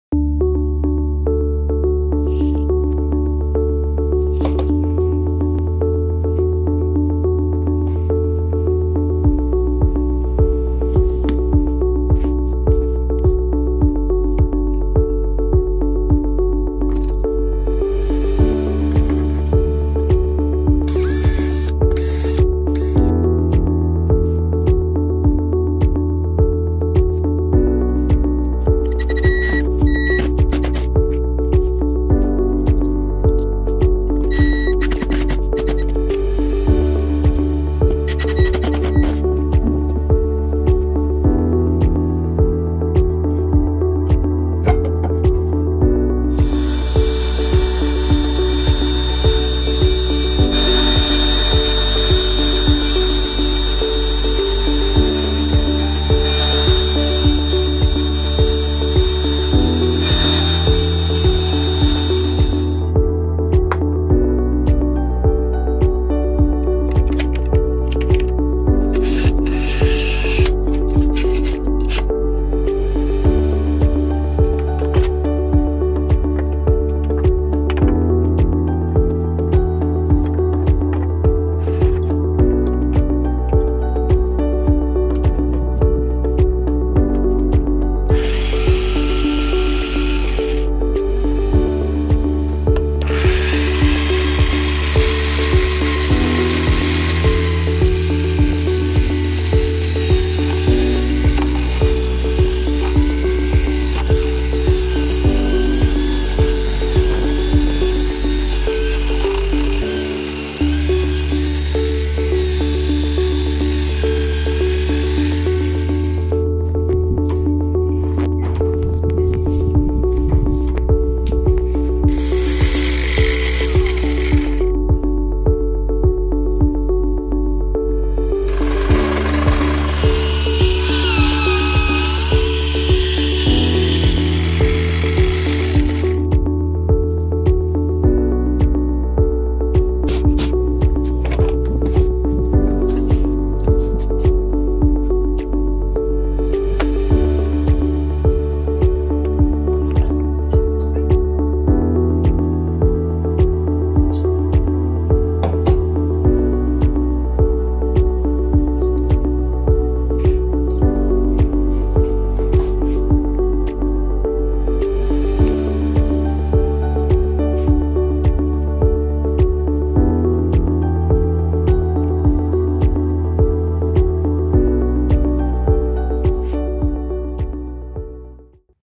Parkside (lidl) lathe wood turning project - Pepper mill holders
A very quick project to make a pepper mill holder. Made using a cheap lidl Parkside lathe machine.